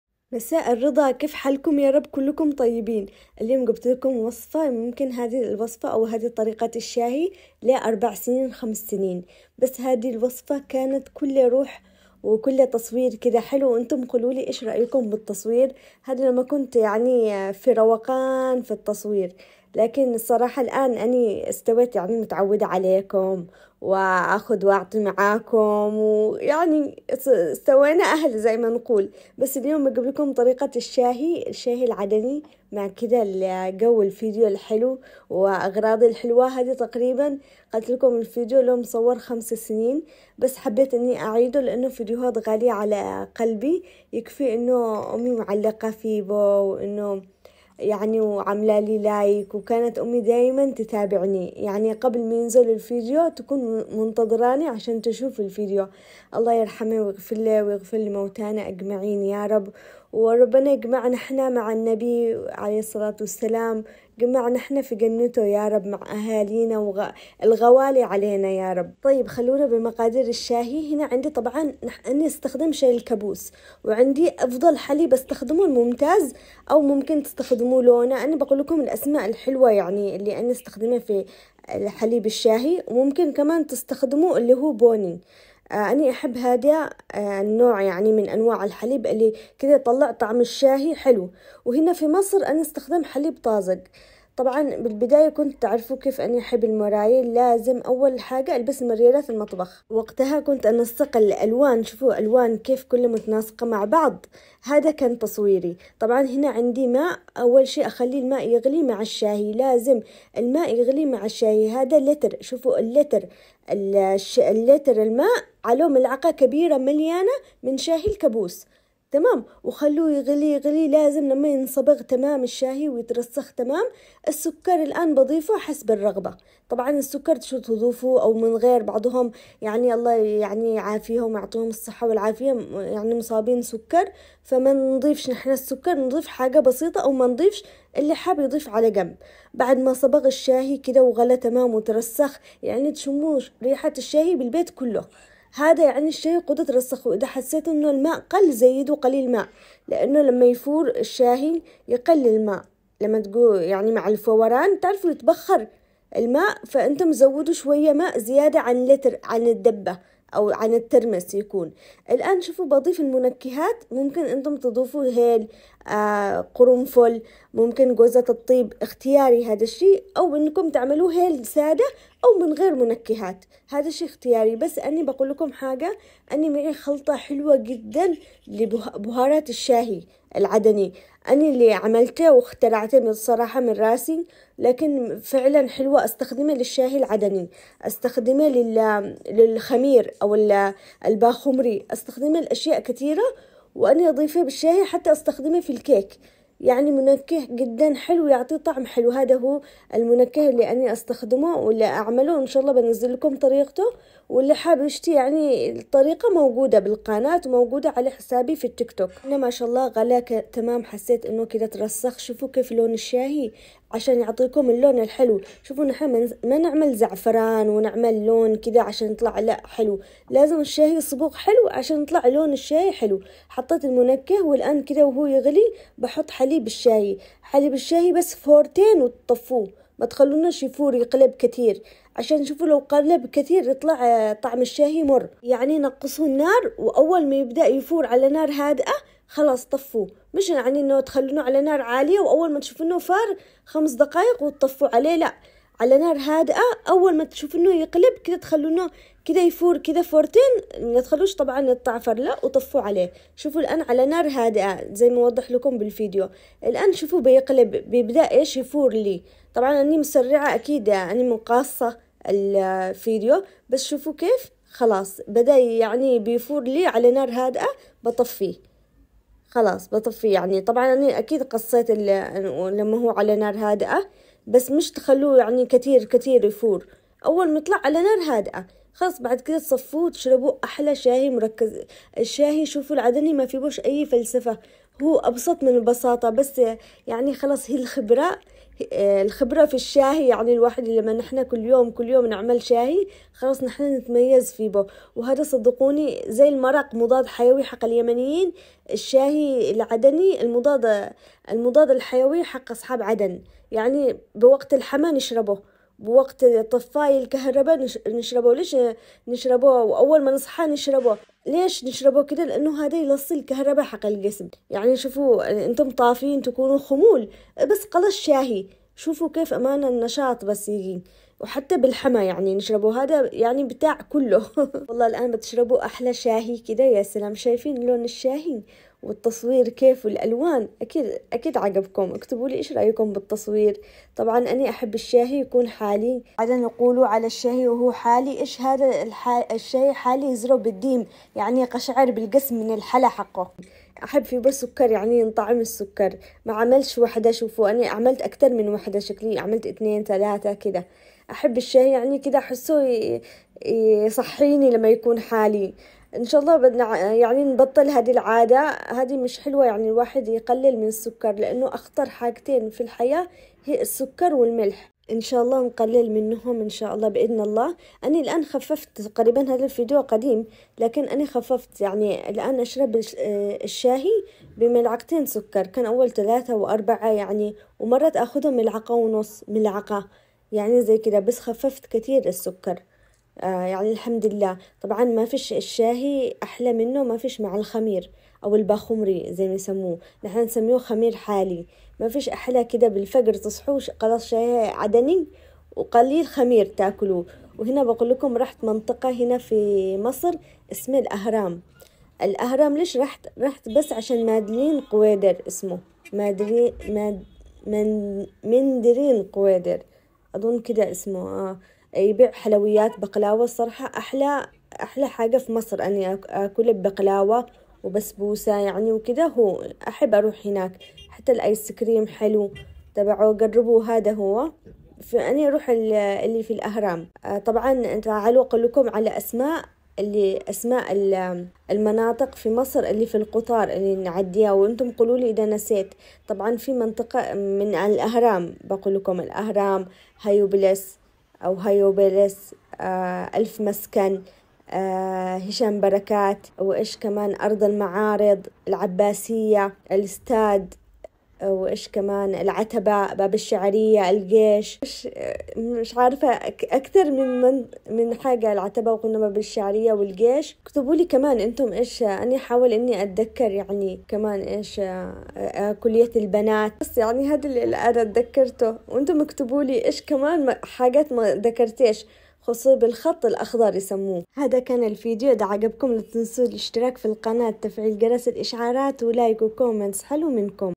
شاي عدني مركز الفديو القريب الى قلبي والقريب زي الغريب
شاي حليب
شاي كرك
شاي عدني
مكونات الشاي العدني
1 لتر ماء
1ملعقة كبيرة شاي الكبوس
سكر حسب الرغبة
علبه حليب مبخر الاممتاز 170 ملي
منكهات هيل او خلطة الشاي العدني